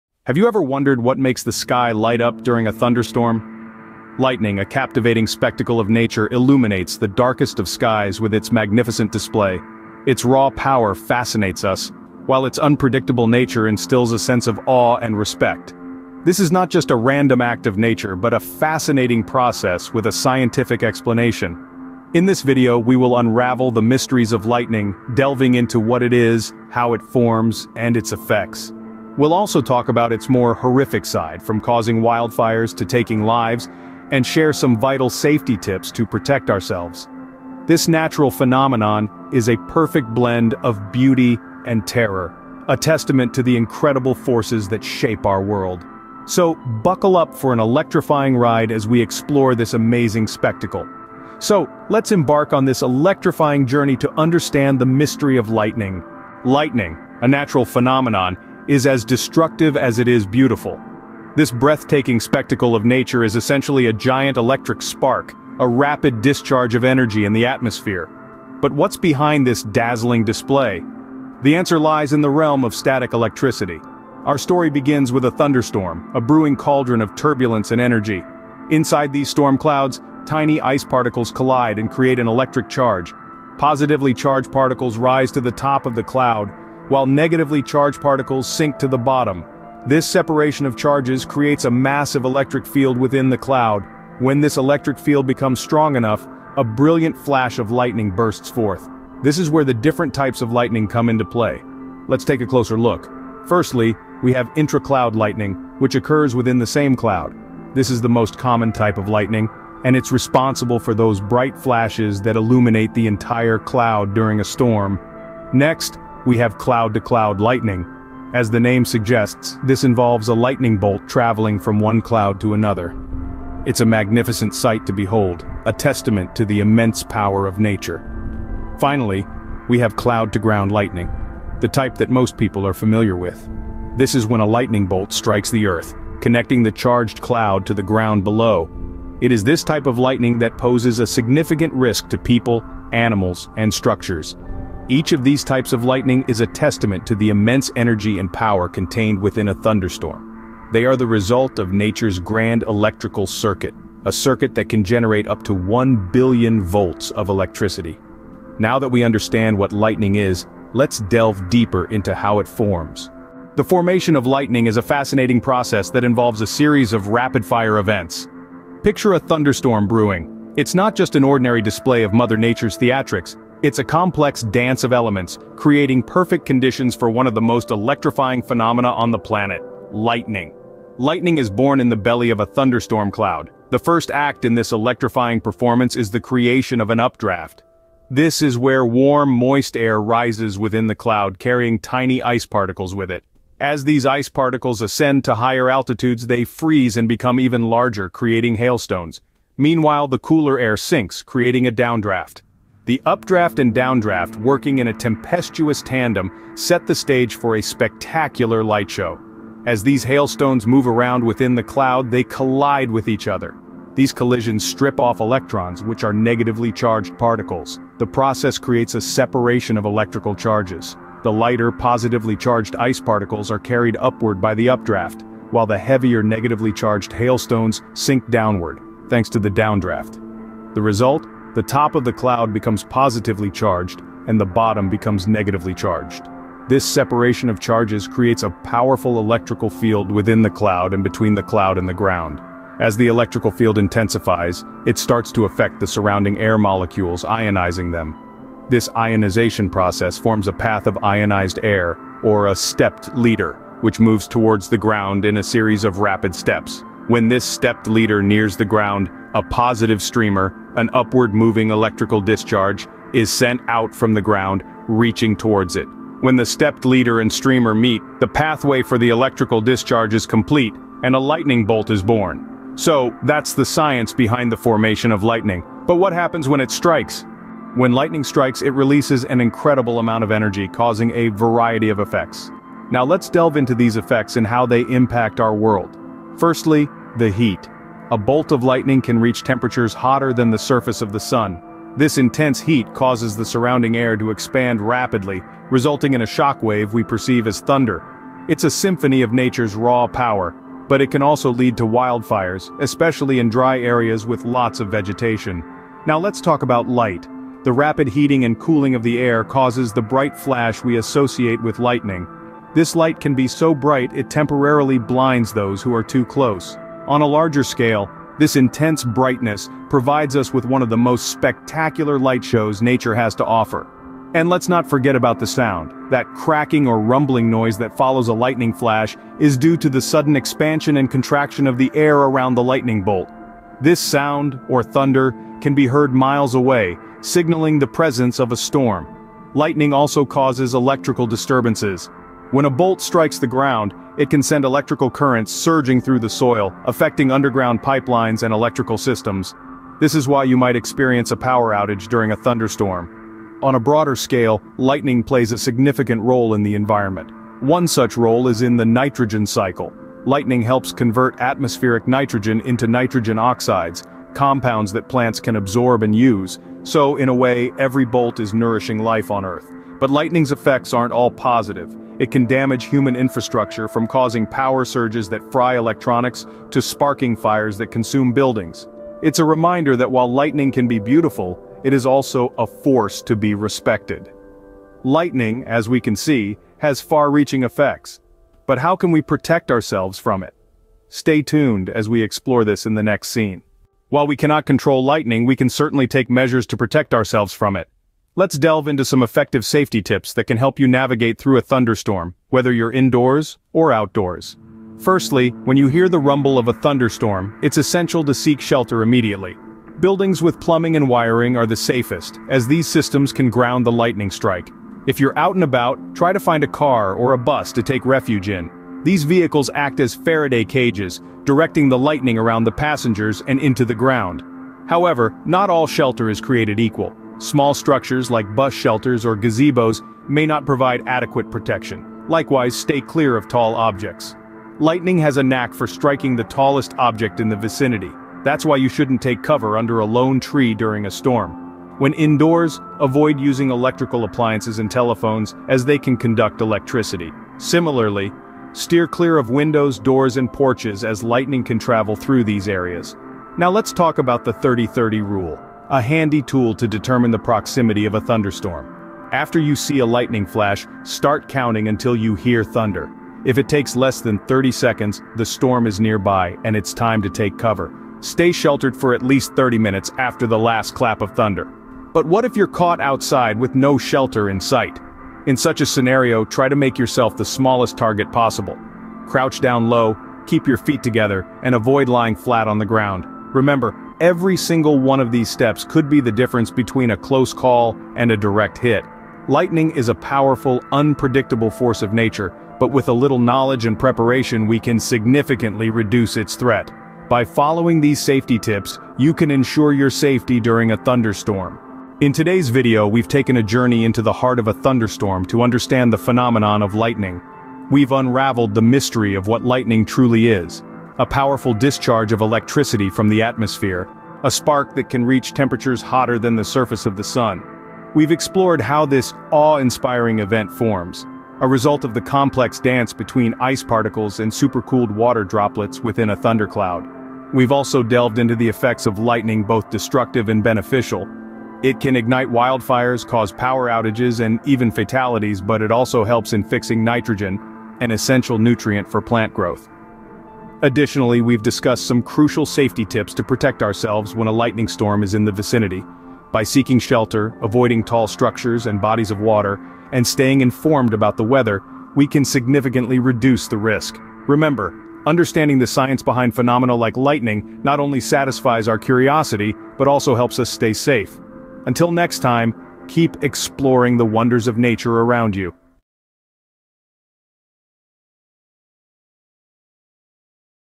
Lightning: Nature's Electric Spectacle
Join us as we delve into the causes behind these awe-inspiring natural phenomena, explore the potential dangers they pose, and most importantly, discover crucial tips on how to stay safe during a lightning storm.

From understanding the science behind lightning to practical safety measures, this video has got you covered. Don't miss out on this enlightening and essential guide to all things lightning!